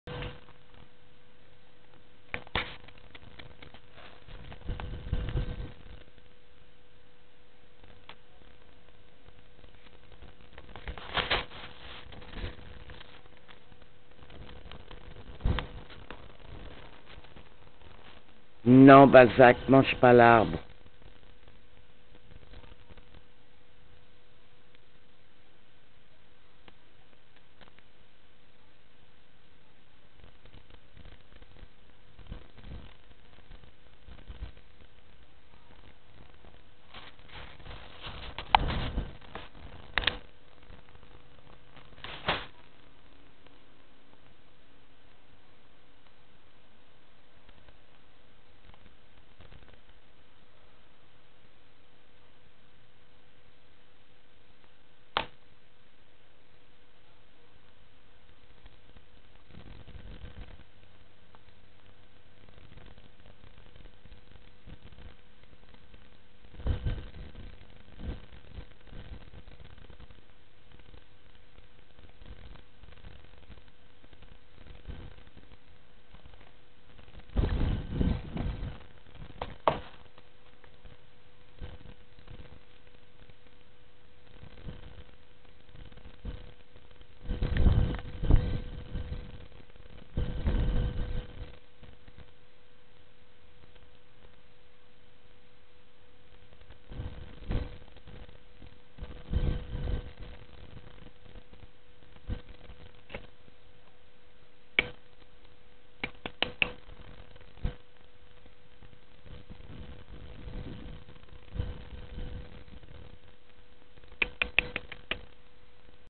Le bucheron
Notre épagneul breton a décider d'émonder à sa manière un cèdre...